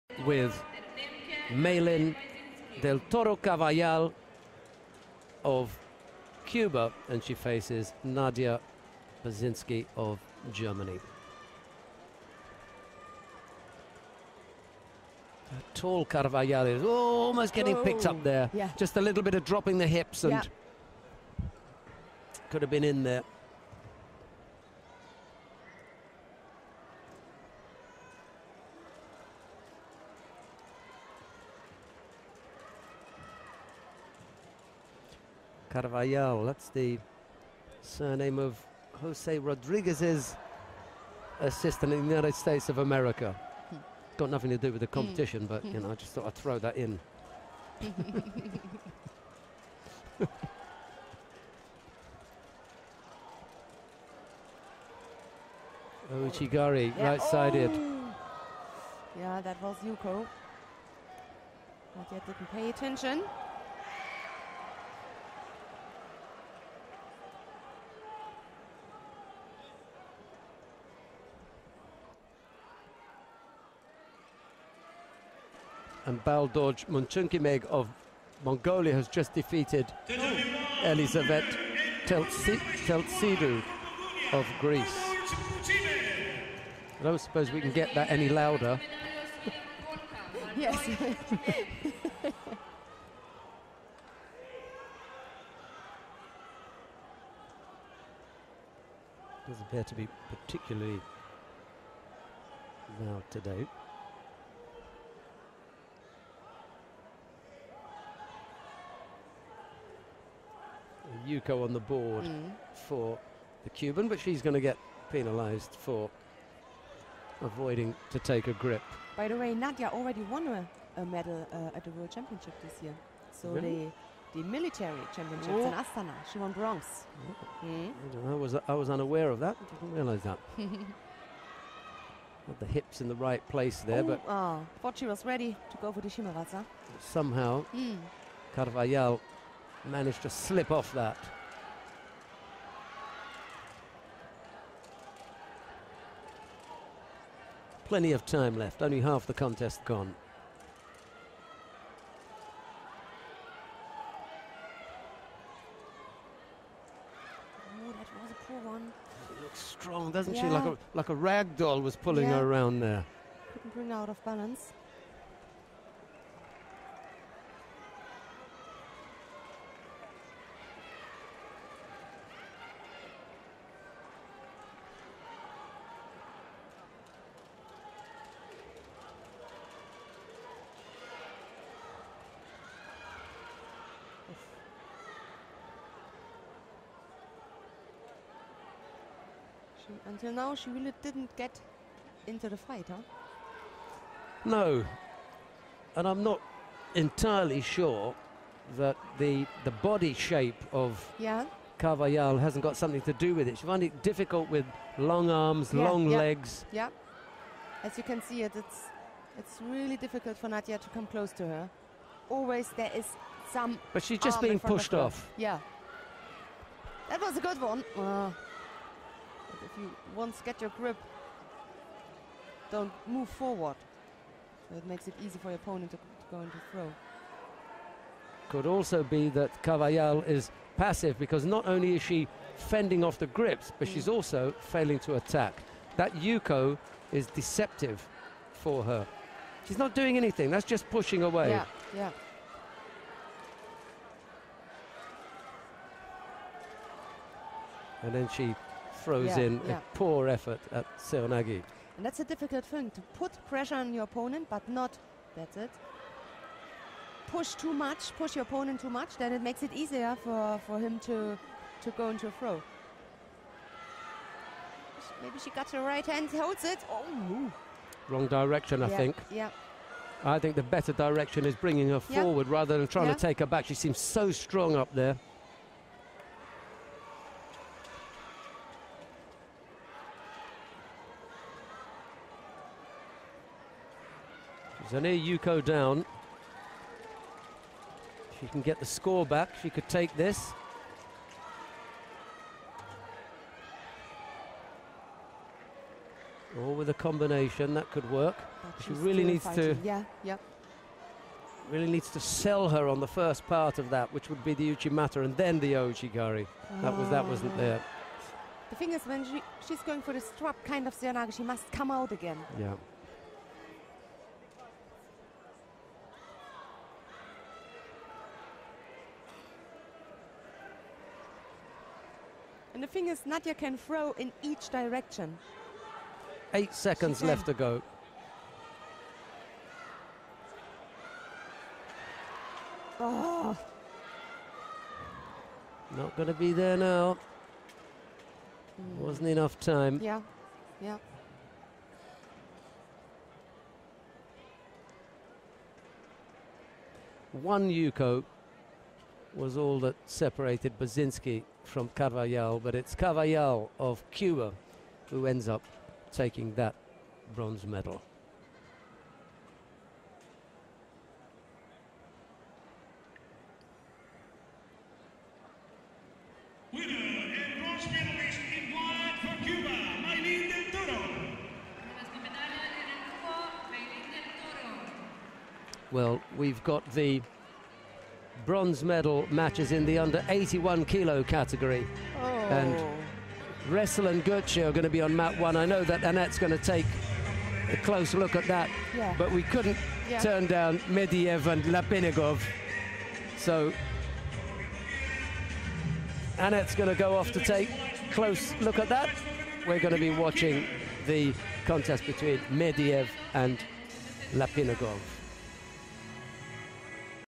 Judo 2013 World Championships Junior: del Toro Carvajal (CUB) - Bazynski (GER) [-63kg] bronze
Maylin del Toro Carvajal (CUB) - Nadja Bazynski (GER) [-63kg] bronze
Judo 2013 World Championships Junior Ljubljana